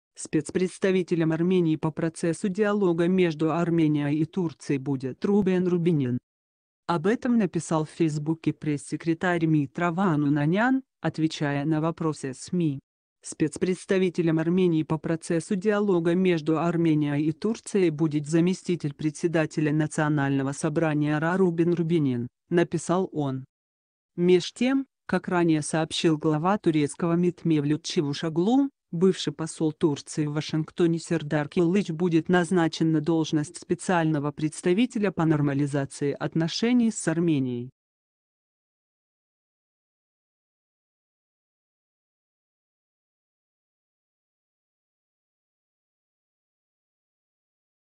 Спецпредставителем Армении по процессу диалога между Арменией и Турцией будет Рубен Рубинян
Об этом написал в фейсбуке пресс-секретарь МИД РА Ваан Унанян, отвечая на вопросы СМИ. «Спецпредставителем Армении по процессу диалога между Арменией и Турцией будет заместитель председателя Национального Собрания РА Рубен Рубинян», - написал он.

Меж тем, как ранее сообщил глава турецкого МИД Мевлют Чавушоглу, бывший посол Турции в Вашингтоне Сердар Кылыч будет назначен на должность специального представителя по нормализации отношений с Арменией.

Армения
Арцах
Карабах
Азербайджан